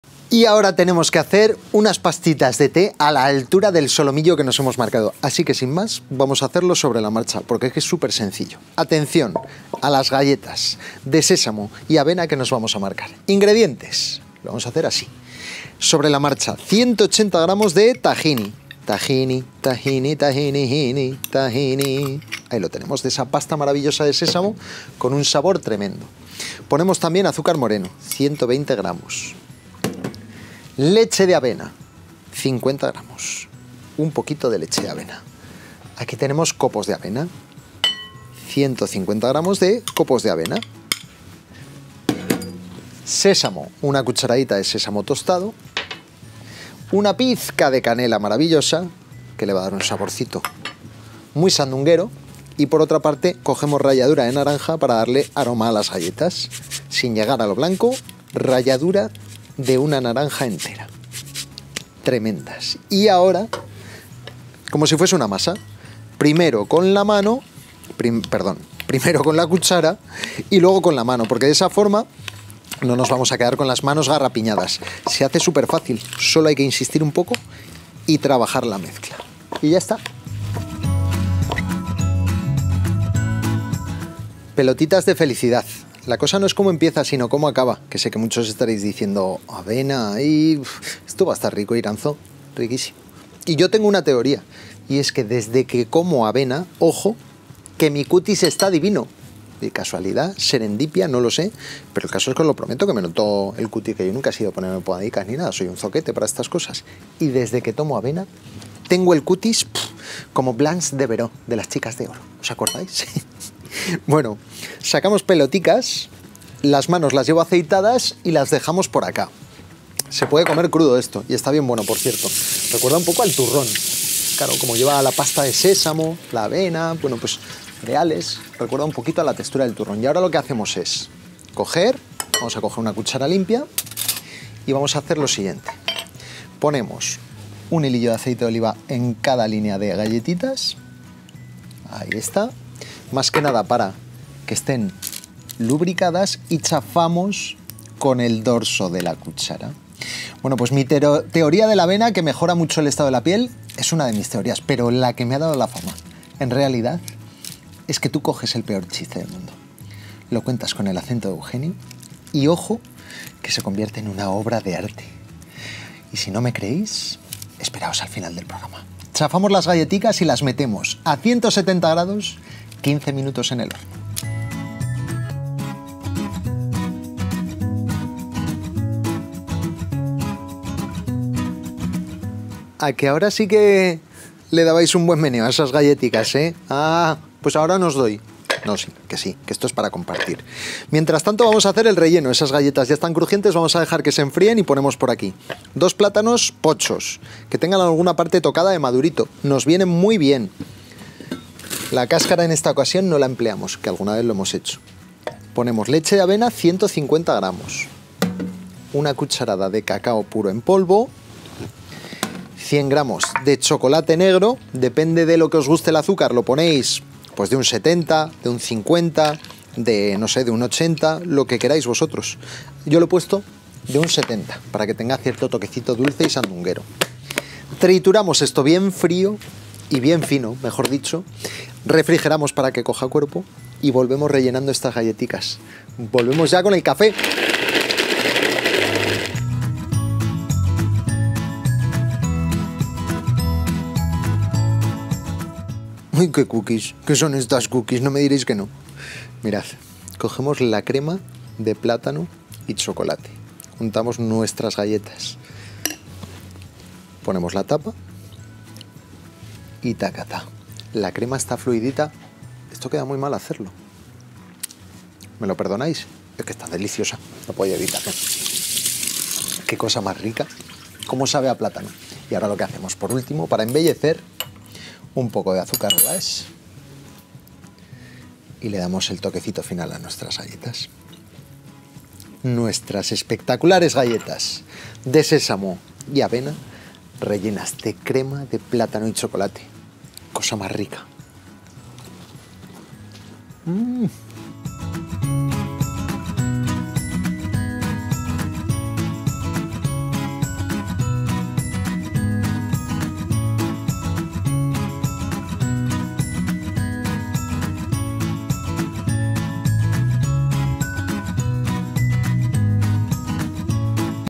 GALLETAS DE AVENA Y SÉSAMO CON CREMA DE CACAO Y PLÁTANO - LPL PGM 2222 RECETA 2
Ingredientes para 4 personas:
150 g de copos de avena, 120 g de azúcar moreno, 180 g de tahini, 1 cuch. de sésamo tostado, 50 g de leche de avena, pizca de canela y ralladura de naranja.
Para rellenar, 2 plátanos maduros, 100 g de chocolate, 150 g de leche de avena y 1 cuch. de cacao en polvo.
Además, azúcar glas.

Elaboración:
Mezclar amasando los ingredientes de las galletas: copos de avena, azúcar moreno, tahini, sésamo tostado, leche de avena, canela y ralladura de naranja. Formar bolitas con las manos aceitadas y chafarlas con la cuchara. Hornear a 170º durante unos 15 minutos, sacar y dejar enfriar.
Mientras, triturar los ingredientes de la crema de plátano: plátanos maduros, chocolate, leche de avena y el cacao en polvo. Dejar enfriar en la nevera.
Rellenar las galletas de crema de plátano y cacao, y servir espolvoreando azúcar glas por encima.